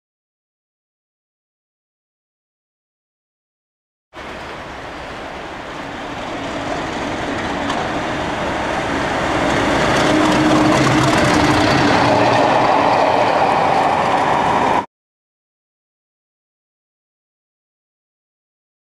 5Z50 50008 Washwood Heath Met.Cammel to Kidderminster S.V.R.
50008 on 5Z50 1355 Washwood Heath Met.Cammel to Kidderminster S.V.R. Seen at Tame Bridge Parkway.